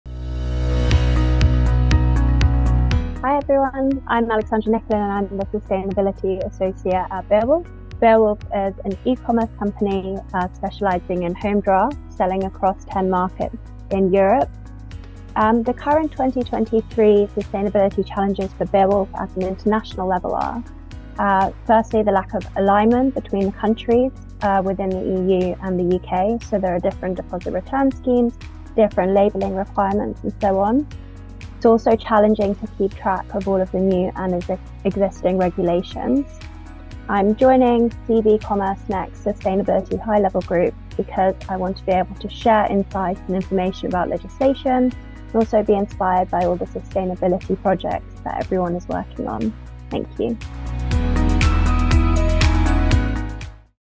CBCommerceNEXT: Beerwulf in Sustainability High Level Group
Welcome to Alexandra Nicolin, Sustainability associate at Beerwulf, that joined our Sustainability High Level Group 2023 edition.

Learn more about her goals in this video.